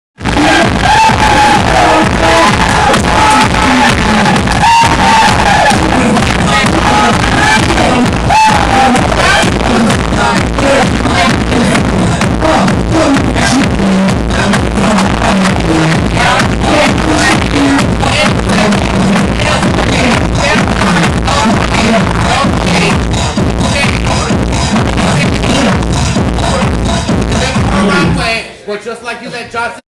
Belucci Miza- 10's- OTA Runway
Battle Zone Ball 3- Mini Series- Miami, Florida-. OTA Runway- Bring it in all white or black with a splash of green and or pink. -Str8 Runway for your 10's, then the shade begins!  The judges wanted Str8 Runway.. But he ate this time regardless. Beluchi Miza- 10's across the board.